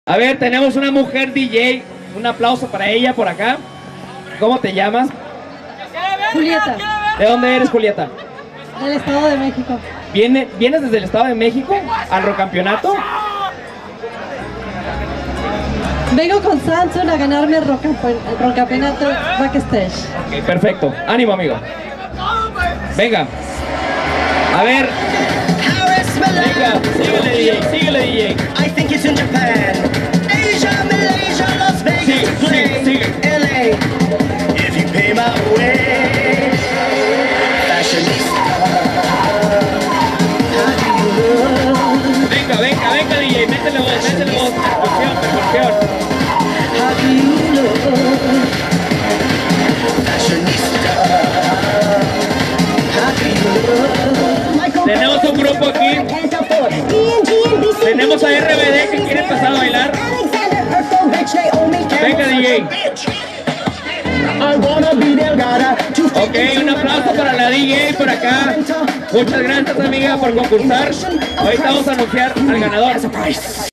P0028 Julieta
Esta es la rola de Julieta que grabo en Hermosillo para participar en la promoción de Beat Edition.

¡¡¡Mucha Suerte!!!